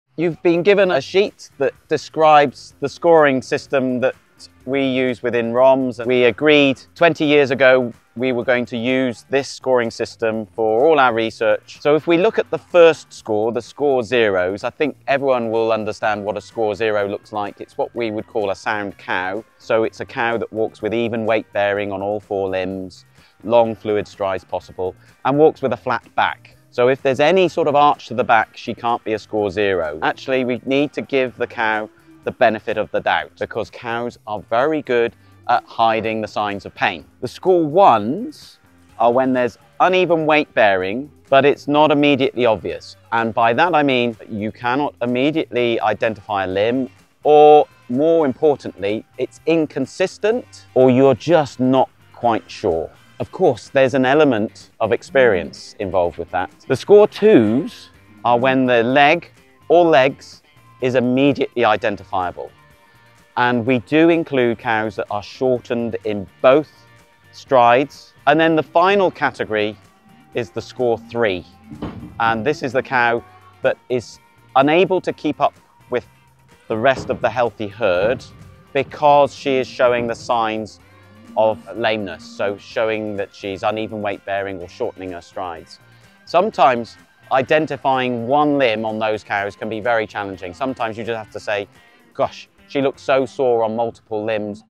Metodo di valutazione
Oggi vediamo il metodo di valutazione che usano tutti i professionisti di settore per valutare le zoppie dei bovini 🐮